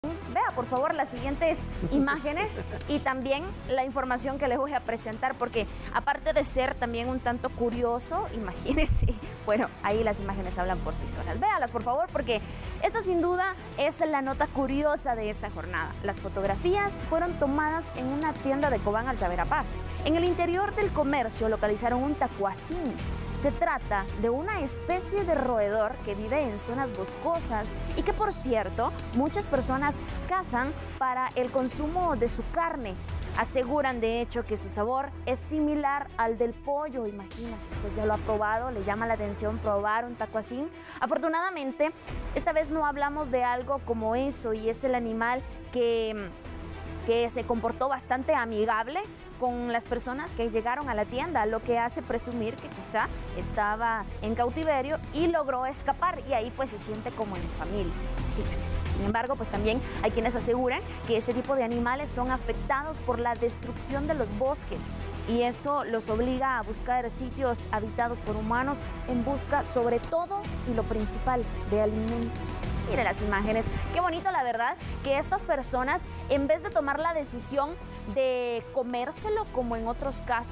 Tacuazín ingresó a tienda de Cobán
Fotografías muestran a un tacuazín en una tienda de Cobán, Alta Verapaz. Especie de roedor que vive en zonas boscosas, el cual es cazado para comer su carne y que sufre las consecuencias de la destrucción de su hábitat. Visita nuestro sitio web